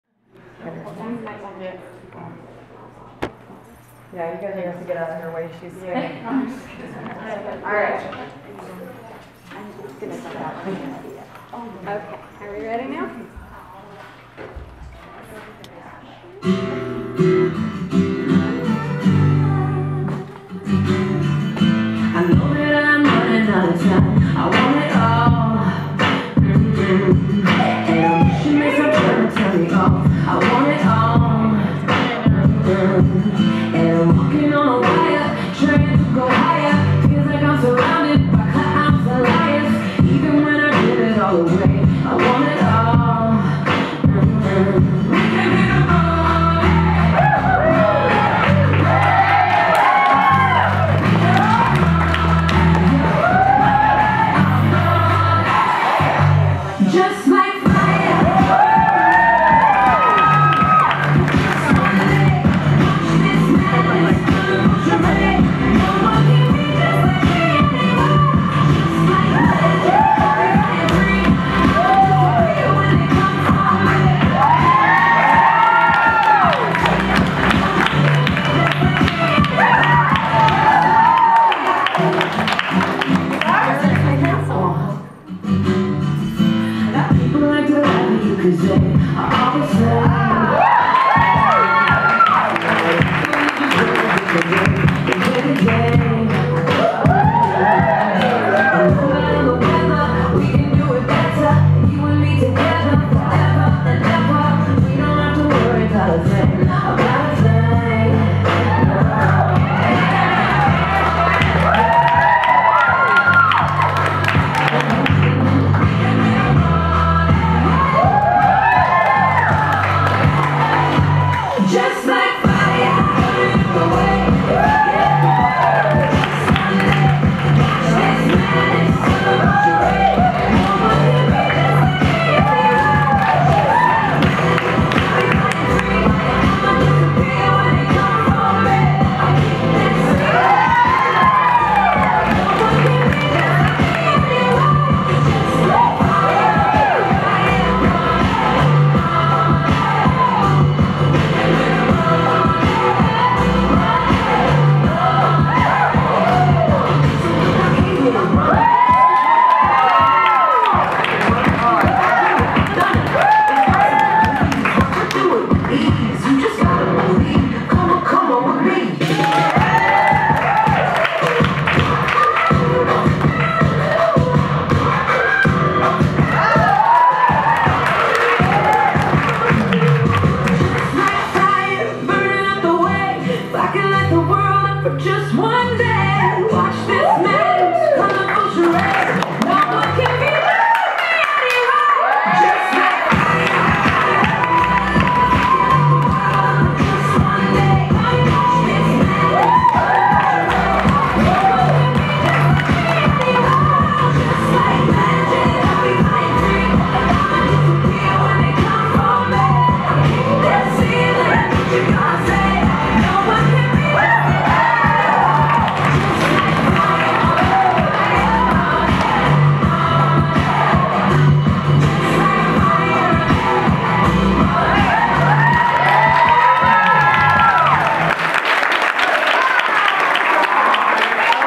Halloween Recital 2016 - FLY KIDS Performance
PoleFIT Revolution's PoleTRICKS & TREATS Halloween 2016 Recital & Party!  Celebrating 2yrs at our 2nd location in Utica, MI.

KIDS love to FLY...Enjoy a fun and energetic performance with students, Chase (age 11) and Gisela (age 8) performing on the hammocks, aerial silks and lyra.  Enroll your kids ages 6-13 for FLY KIDS classes let them get FIT & FLY with this circus style class.